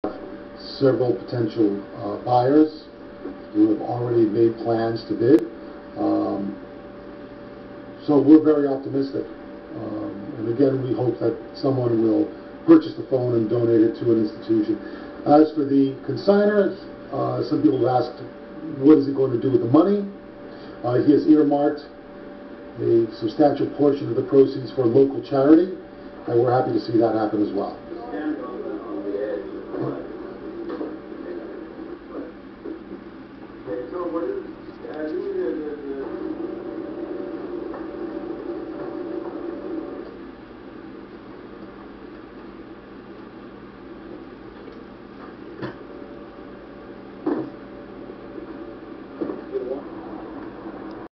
MOV02794 {{ Hitler's Office Phone Auction Off - 2009- }}}
Hitler's Office Phone was Auction off for : $240,000.00 by Wheatcrolt . I like to know WHY COLOR IS COMING OFF THE PHONE . I can see HITLER keeping Terrible Looking  PHONE IN HIS OFFICE . I think Twice Before I bet On IT . It Does Not LOOK LIKE A PHONE HITLER Would Use In His OFFICE